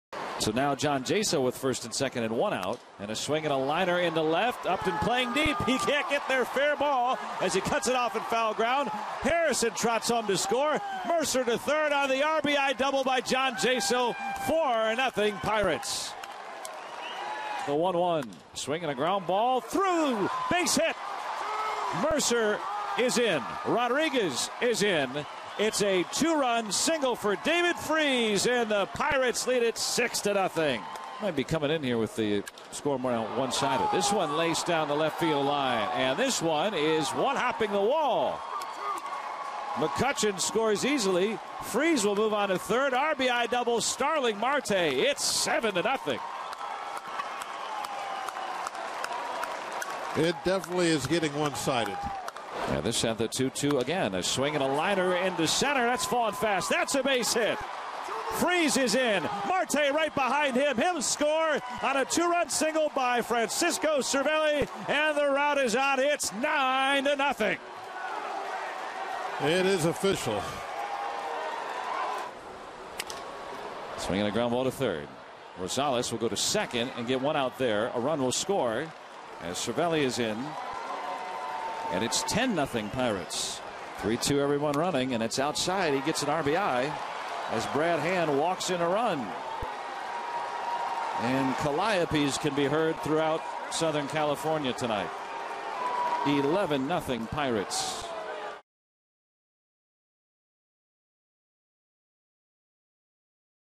PIT@SD: Pirates score eight runs in the 7th inning
The Pirates do eight runs of damage in the top of the 7th innning, with six different players collecting RBIs during the frame